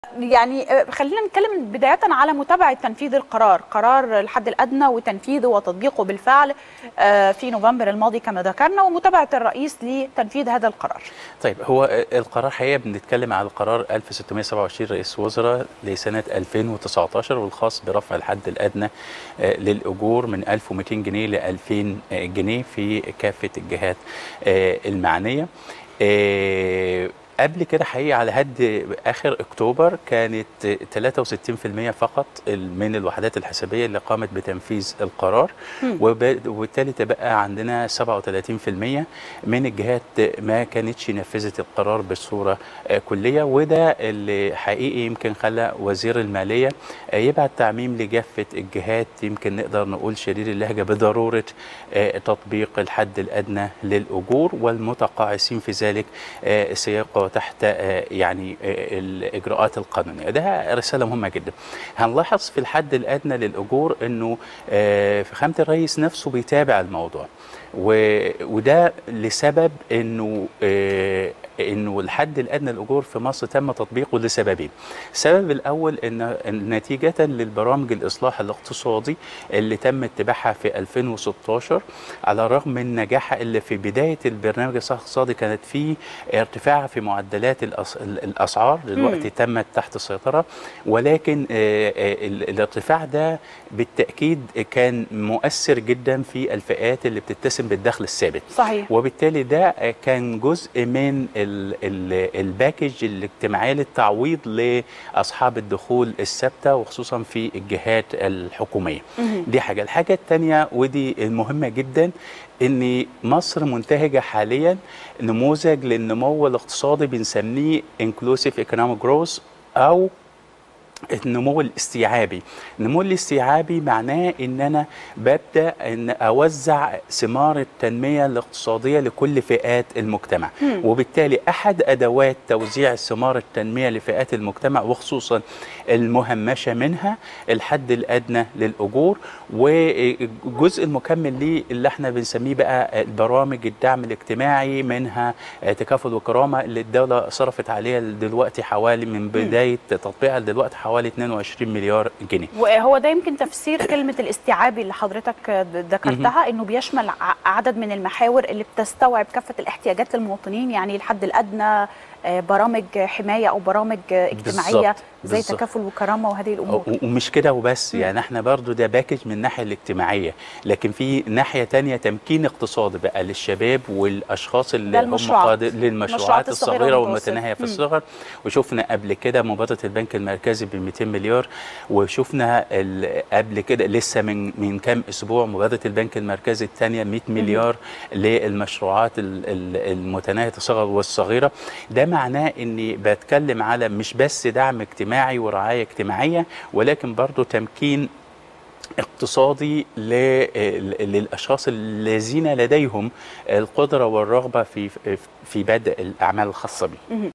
الآن | أستاذ اقتصاد: ارتفاع معدل النمو الاقتصادي ساعد الحكومة على تطبيق الحد الأدنى للأجور
الآن | أستاذ اقتصاد: ارتفاع معدل النمو الاقتصادي ساعد الحكومة على تطبيق الحد الأدنى للأجور | 22 ديسمبر 2019